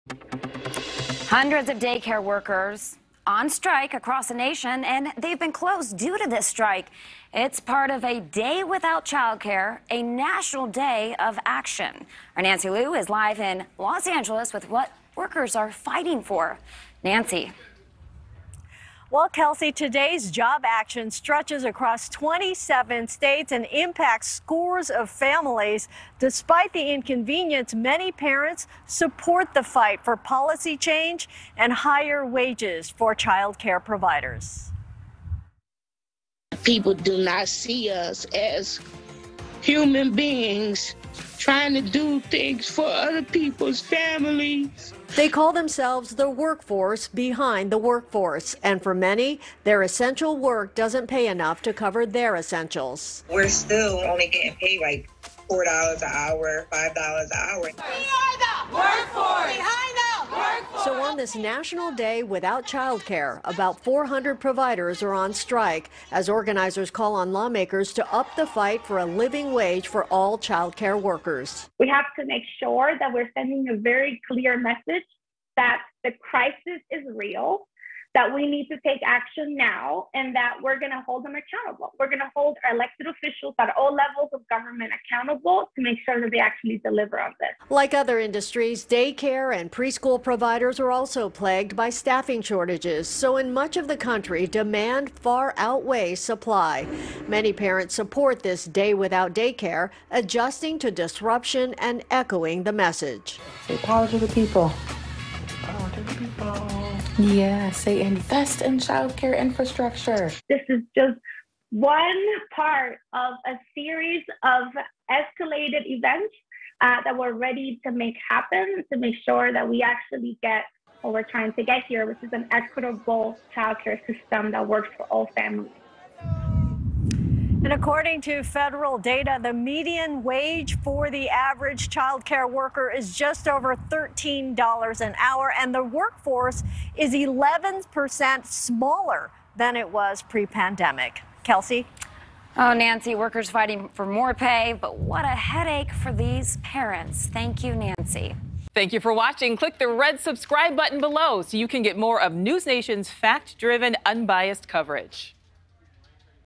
Hundreds of day care workers are on strike across the nation | Rush Hour
Hundreds of day care centers across the country were closed on Monday as workers went on strike for better wages. NewsNation's Nancy Loo broke down the strike on NewsNation Prime.